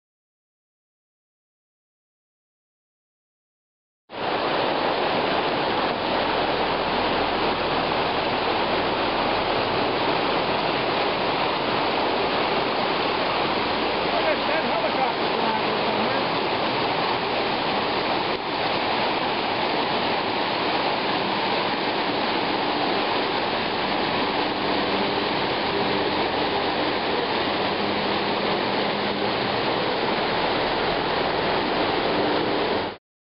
Dry Branch in Flood
The Dry Branch of the Elk River, bone dry when we went to sleep, was in raging flood when we awoke in the morning! With a drainage basin of 92-square-miles, this karst area is extremely flashy; we're lucky that we were in camp when the flash flood occurred rather than underground!